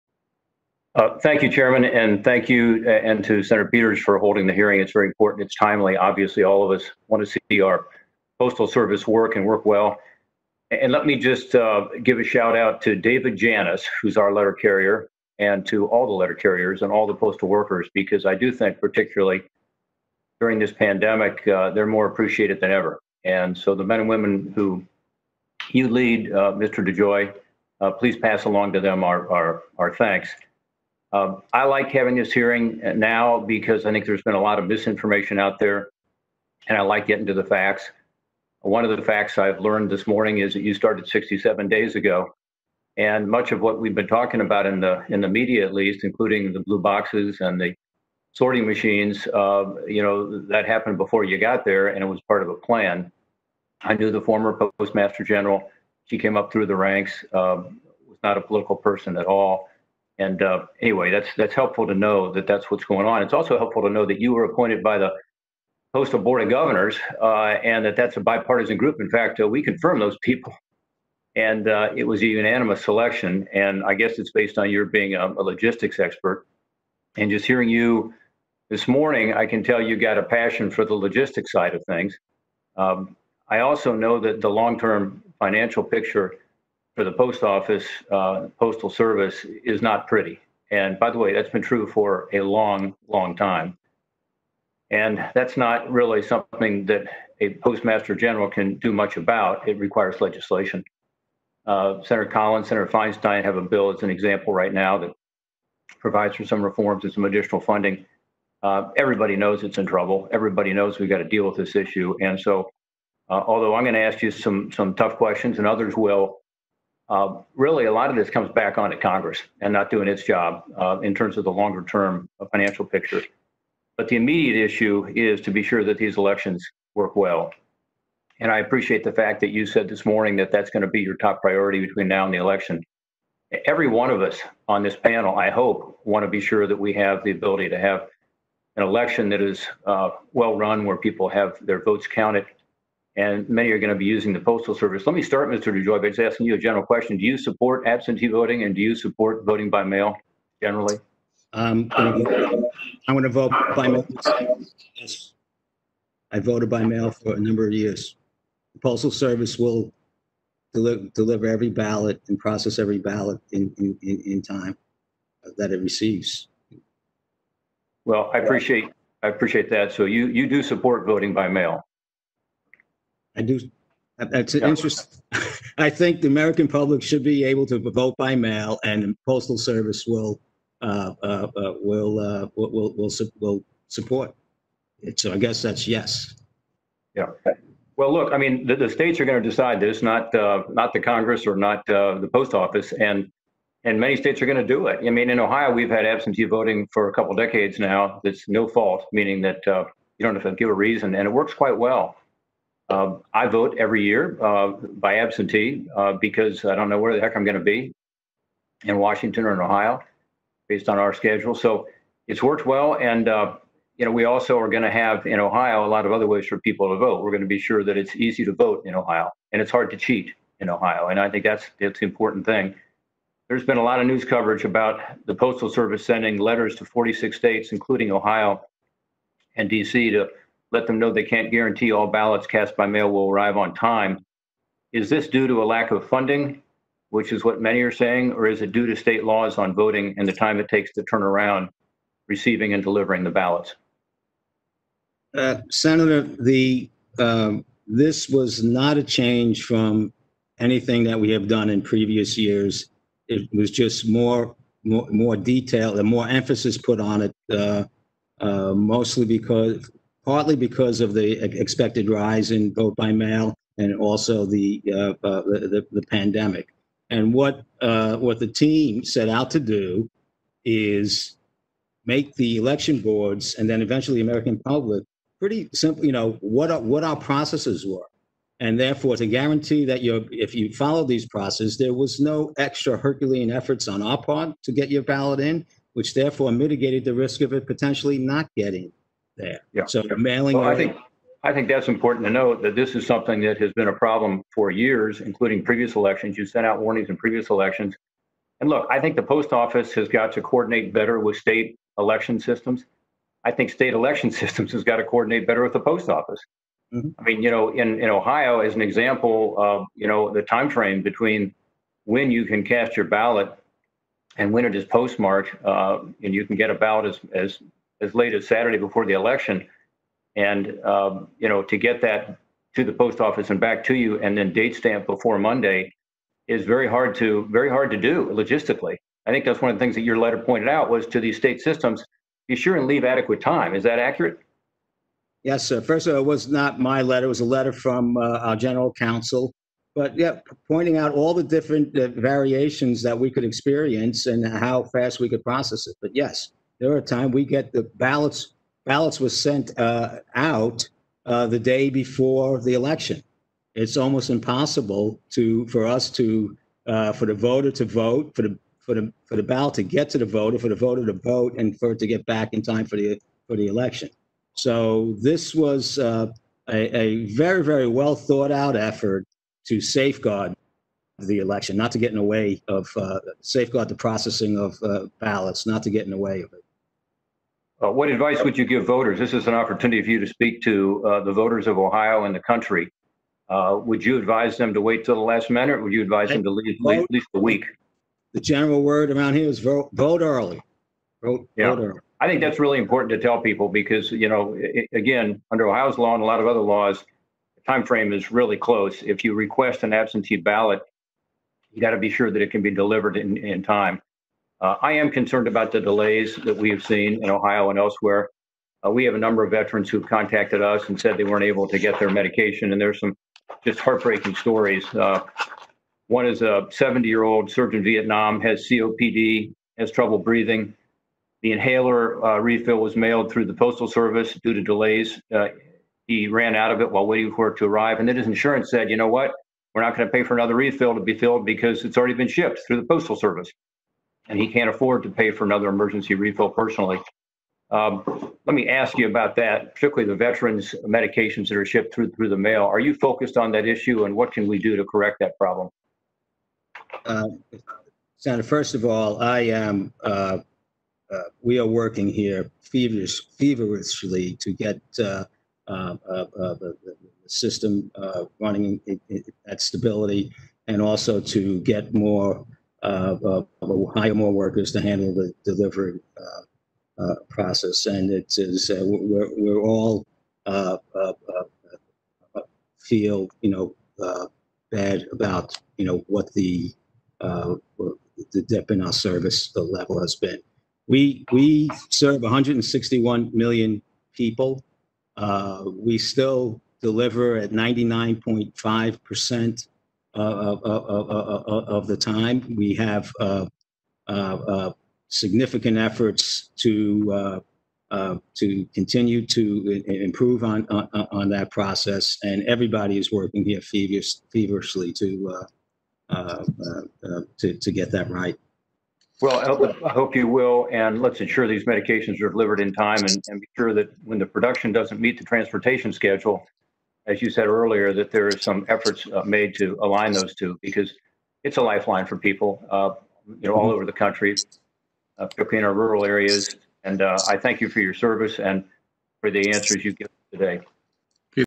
Sen. Rob Portman, R., Ohio questions USPS Postmaster General Louis DeJoy
The senator said that he has heard from a number of veterans expressing concern about delays in the United States Postal Service delivery of their critical prescription medications. "This is not acceptable," Portman says.